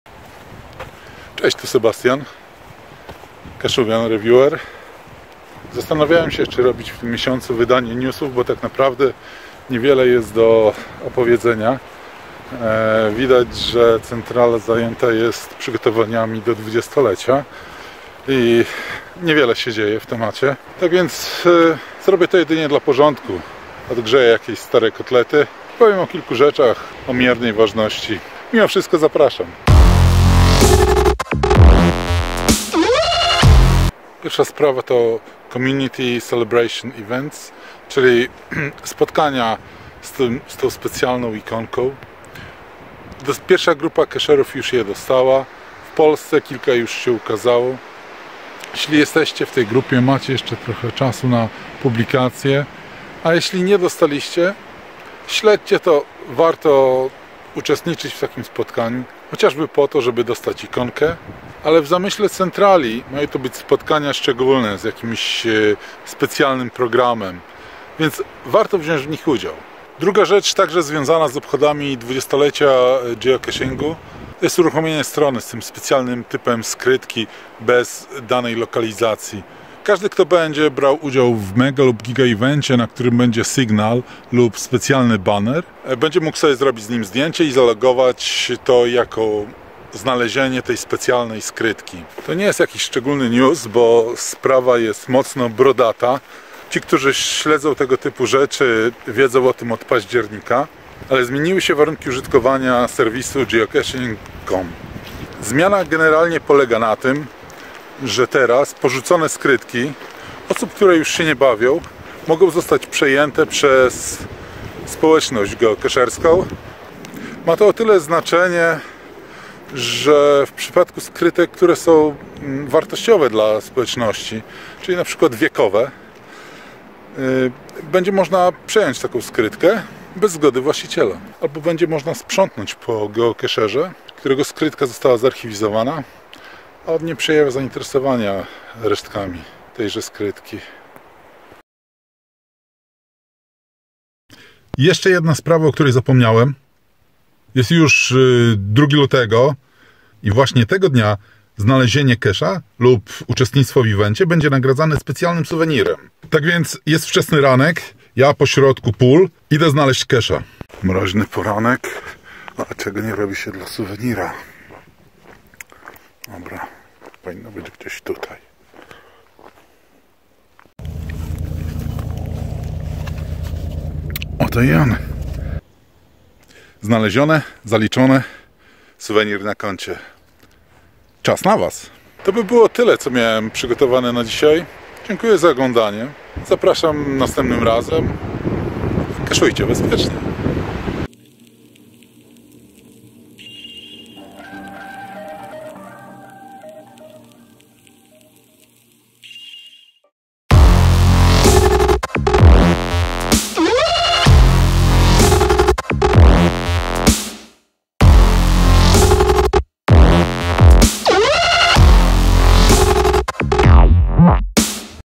A wiecie, że... (Luty 2020) - wiadomości geocachingowe
W dzisiejszym odcinku:

00:35 * Obchody 20-lecia geocachingu na eventach organizowanych przez geocacherów

01:10 * Locationless geocache

01:32 * O aktualizacji Zasad Użytkowania (Terms of Use)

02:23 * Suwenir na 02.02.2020

03:10 * Zapowiedź chatu LIVE

Keszujcie bezpiecznie!
Sebastian / Kashubian Reviewer